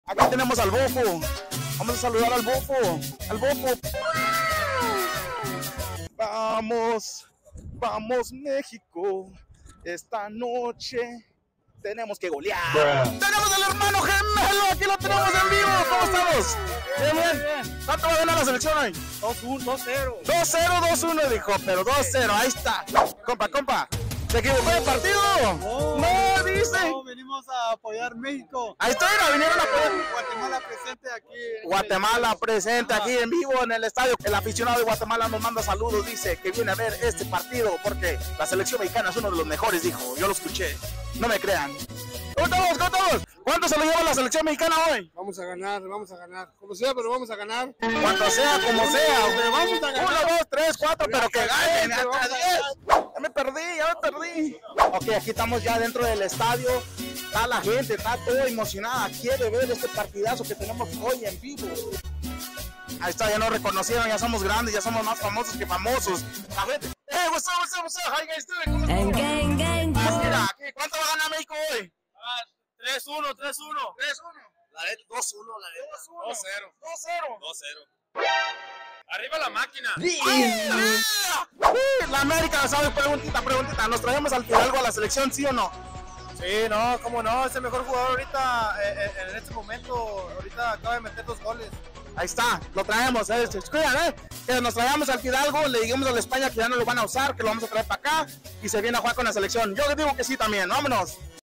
TALKING TO THE FANS USA VS MEXICO STATE FARM ARIZONA W/S4DAVES
New videos Monday Wednesday and Fridays!!

Spanglish Content!